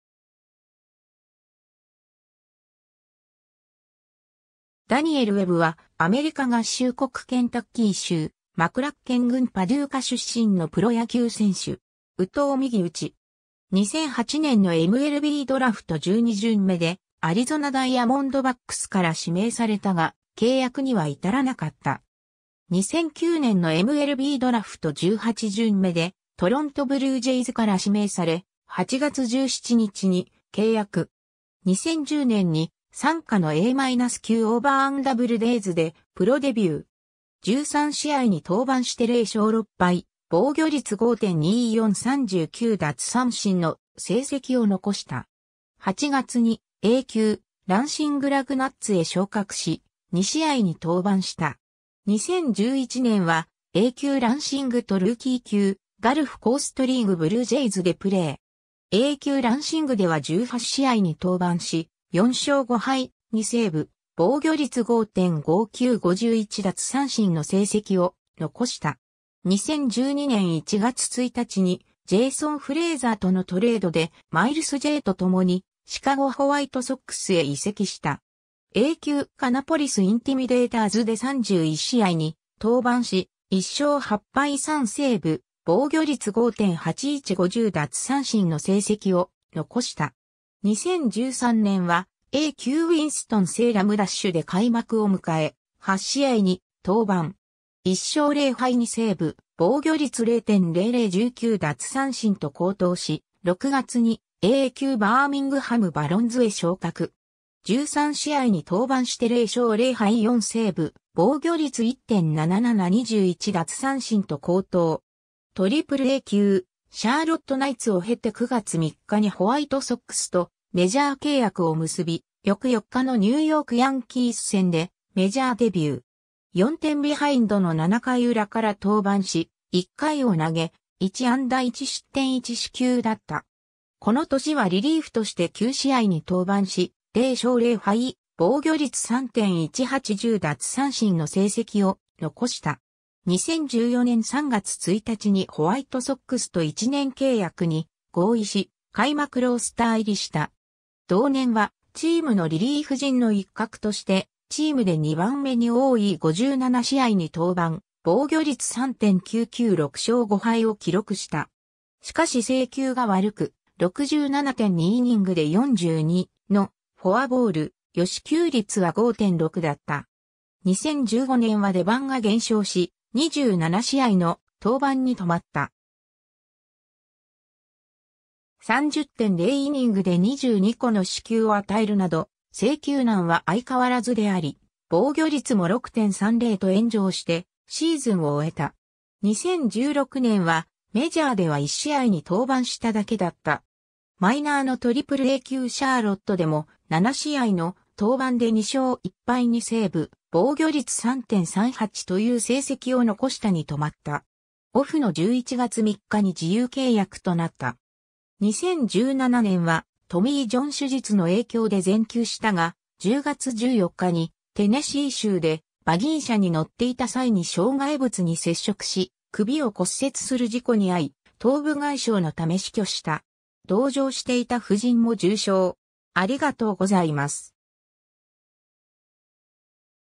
ダニエル・ウェブ (野球)
ダニエル・ウェブ（Daniel Webb、本名：ロバート・マクダニエル・ウェブ, Robert McDaniel Webb, 1989年8月18日 - 2017年10月14日）は、アメリカ合衆国ケンタッキー州マクラッケン郡パデューカ出身のプロ野球選手（投手）。
右投右打。
 2008年のMLBドラフト12巡目（全体378位）でアリゾナ・ダイヤモンドバックスから指名されたが、契約には至らなかった。
 2009年のMLBドラフト18巡目（全体550位）でトロント・ブルージェイズから指名され、8月17日に契約。
 2010年に傘下のA-級オーバーン・ダブルデイズでプロデビュー。
13試合に登板して0勝6敗・防御率5.24・39奪三振の成績を残した。
8月にA級ランシング・ラグナッツへ昇格し、2試合に登板した。
 2011年はA級ランシングとルーキー級ガルフ・コーストリーグ・ブルージェイズでプレー。
A級ランシングでは18試合に登板し、4勝5敗2セーブ・防御率5.59・51奪三振の成績を残した。
 2012年1月1日にジェイソン・フレイザーとのトレードで、マイルス・ジェイと共にシカゴ・ホワイトソックスへ移籍した。
A級カナポリス・インティミデイターズで31試合に登板し、1勝8敗3セーブ・防御率5.81・50奪三振の成績を残した。
 2013年はA+級ウィンストン・セイラム・ダッシュで開幕を迎え、8試合に登板。
1勝0敗2セーブ・防御率0.00・19奪三振と好投し、6月にAA級バーミングハム・バロンズへ昇格。
13試合に登板して0勝0敗4セーブ・防御率1.77・21奪三振と好投。
AAA級シャーロット・ナイツを経て9月3日にホワイトソックスとメジャー契約を結び、翌4日のニューヨーク・ヤンキース戦でメジャーデビュー。
4点ビハインドの7回裏から登板し、1回を投げ1安打1失点1四球だった。
この年はリリーフとして9試合に登板し、0勝0敗・防御率3.18・10奪三振の成績を残した。
 2014年3月1日にホワイトソックスと1年契約に合意し、開幕ロースター入りした。
同年はチームのリリーフ陣の一角として、チームで2番目に多い57試合に登板、防御率3.99・6勝5敗を記録した。
しかし制球が悪く、67.2イニングで42のフォアボール・与四球率は5.6だった。
 2015年は出番が減少し、27試合の登板に留まった。
30.0イニングで22個の四球を与えるなど、制球難は相変わらずであり、防御率も6.30と炎上してシーズンを終えた。
 2016年は、メジャーでは1試合に登板しただけだった。
マイナーのAAA級シャーロットでも、7試合の登板で2勝1敗2セーブ・防御率3.38という成績を残したに留まった。
オフの11月3日に自由契約となった。
 2017年はトミー・ジョン手術の影響で全休したが、10月14日にテネシー州でバギー車に乗っていた際に障害物に接触し、首を骨折する事故に遭い、頭部外傷のため死去した。
同乗していた夫人も重傷。